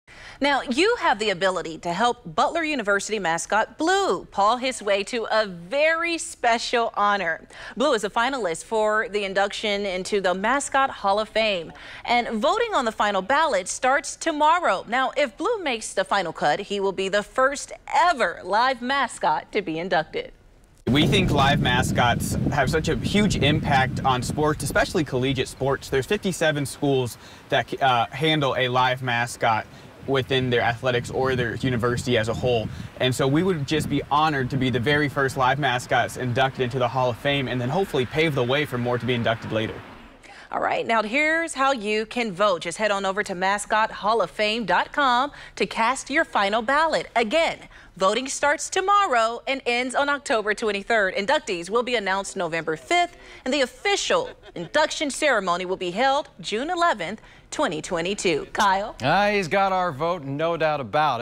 Butler Blue is a finalist for the Mascot Hall of Fame
Butler Blue could be the first live mascot inducted into the Mascot Hall of Fame- but he needs your votes!